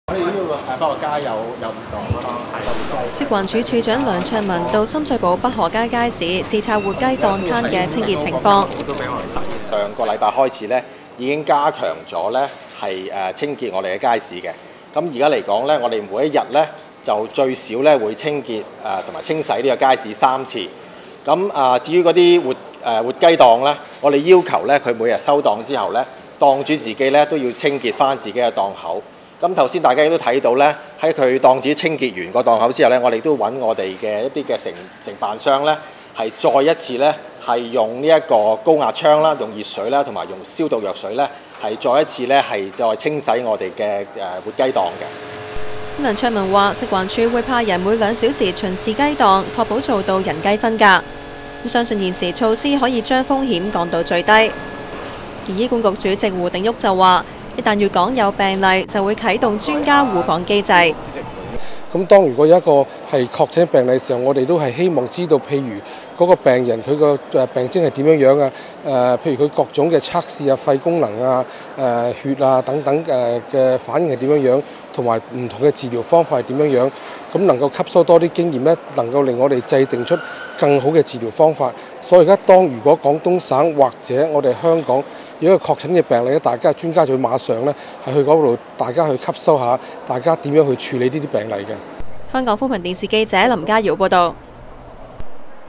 粵港專家互訪抗H7N9
內地感染H7N9禽流感人數持續上升，醫管局主席胡定旭表示，如果粵港其中一方有病例，另一方會馬上派專家吸取臨床診斷經驗。而食環署署長梁卓文，就去街市視察雞販清潔情況。